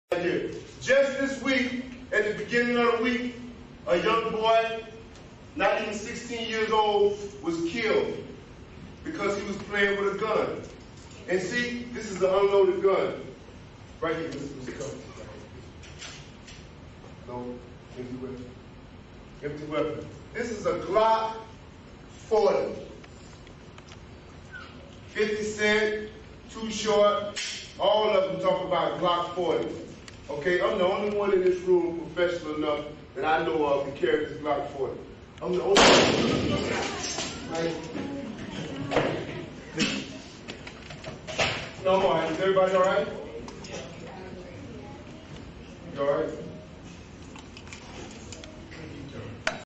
Federal Agent Accidentally Shot Himself
This video went viral in 2004 after the DEA agent Lee Paige accidentally shot himself while lecturing children on gun safety.

He was suspended for five days without pay after the accident, and the video was turned over to the Drug Enforcement Administration.

Paige sued the Drug Enforcement Agency for releasing the video to the public, thereby violating his privacy and ended his ability to work undercover or give motivational speeches.

in December 2010, a U.S. District court judge ruled that Paige had provided no evidence of who made the video public, and a three-judge appeals court panel upheld that ruling and found that the video contained no private facts because the accidental discharge occurred in an Orlando community center at an event open to the public.

After Paige shot himself, he tried to continue his presentation and said: "See how that accident happened?" he told the audience. "It can happen to you, and you can be blown away. So, guys, never play with guns."